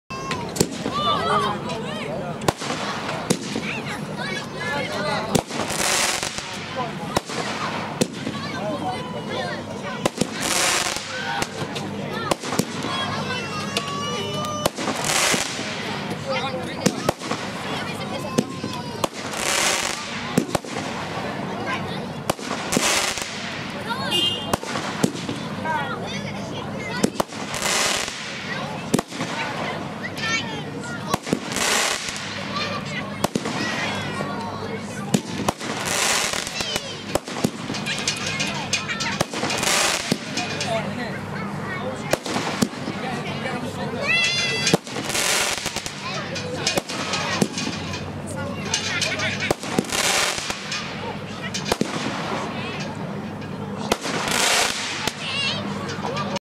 hayleys comet - benstar fireworks
filmed and fired by CP fireworks at the 2013 pyrofix